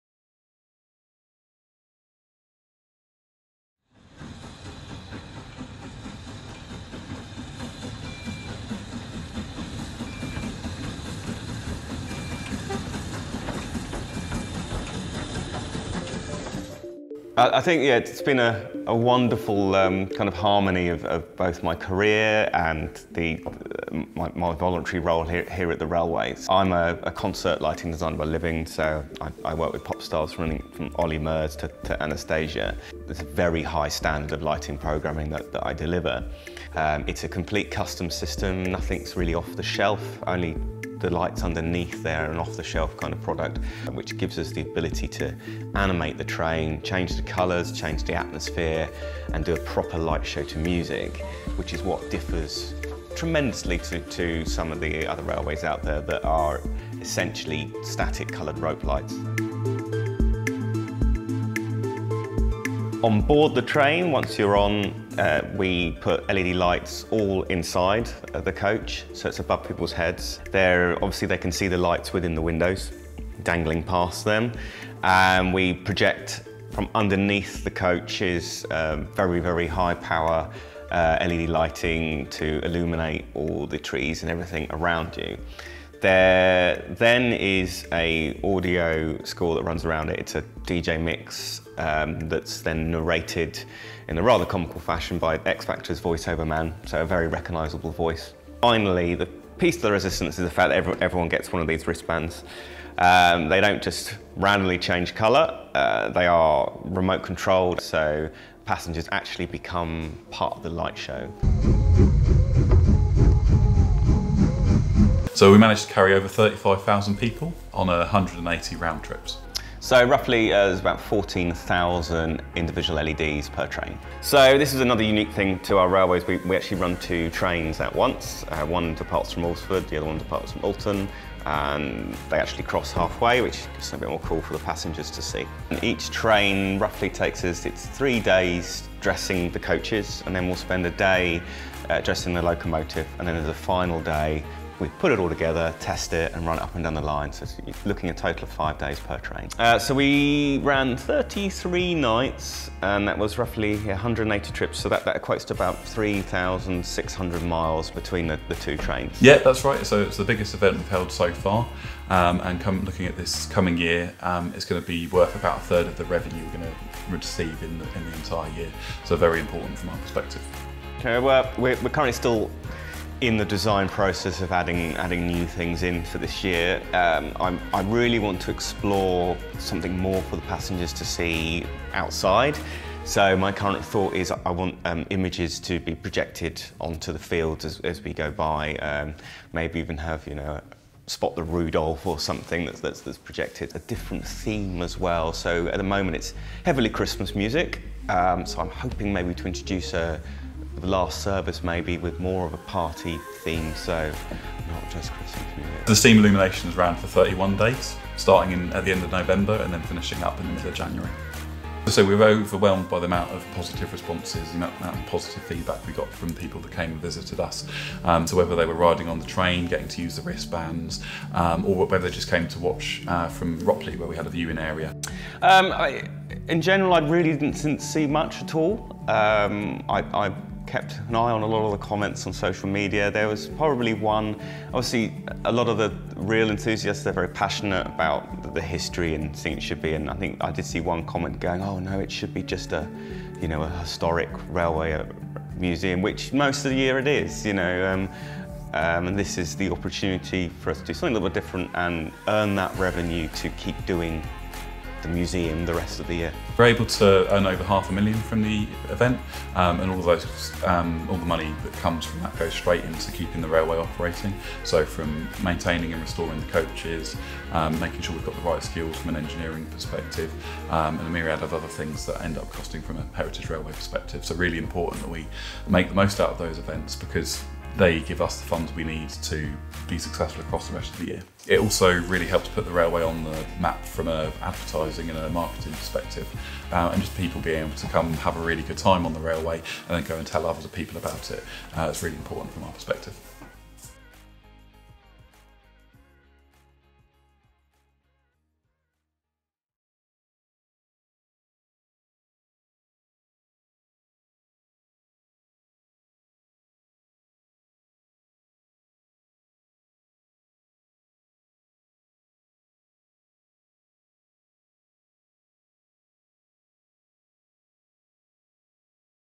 Steam Illuminations - Things to Know
A video about the Steam Illuminations I made for The Midhants Railway's Watercress Line as part of my Video Production Module at Solent University.